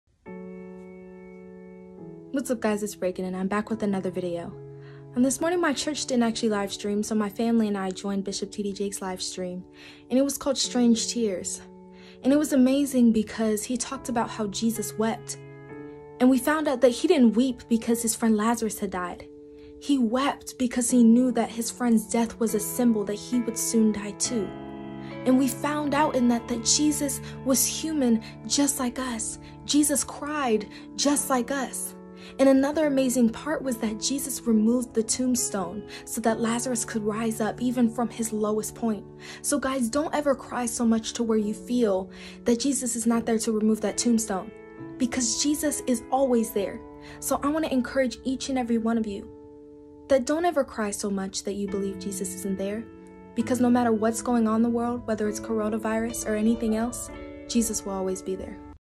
Strange Tears- Bishop TD Jake Motivational speaker and child published author Reagan Nevels
Motivational speaker/child published author Reagan Nevels is inspired by Bishop Td Jakes Message!